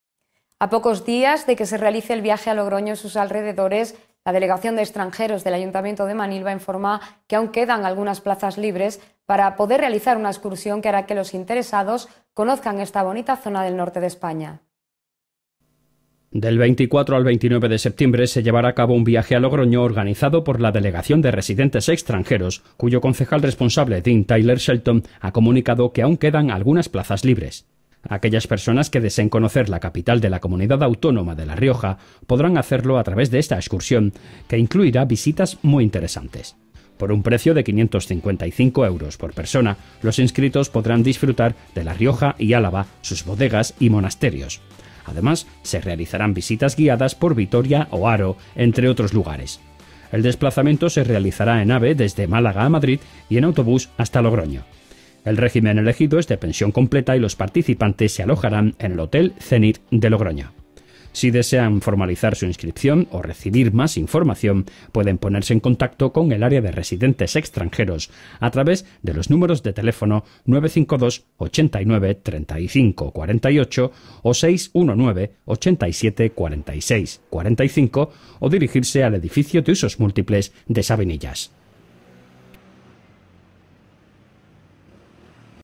AÚN QUEDAN PLAZAS PARA EL VIAJE A LOGROÑO ORGANIZADO POR EXTRANJEROS
Del 24 al 29 de septiembre se llevará a cabo un viaje a Logroño organizado por la Delegación de Residentes Extranjeros, cuyo concejal responsable Dean Tyler Shelton, ha comunicado que aún  quedan algunas plazas libres.
Aquellas personas que deseen conocer la capital de la comunidad autónoma de La Rioja podrán hacerlo a través de esta excursión que incluirá visitas muy interesantes.
Por un precio de 555 euros por persona, los inscritos podrán disfrutar de La Rioja y Álava, sus bodegas y monasterios. Además, se realizarán visitas guiadas por Vitoria o Haro, entre otros lugares.
El desplazamiento se realizará en AVE desde Málaga a Madrid y en autobús hasta Logroño.
El régimen elegido es de pensión completa y los participantes se alojarán en el Hotel Zenit de Logroño.
Si desean formalizar su inscripción o recibir más información pueden ponerse en contacto con el área de Residentes Extranjeros a través de los números de teléfono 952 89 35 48 ó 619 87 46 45 o dirigirse al edificio del usos múltiples en Sabinillas.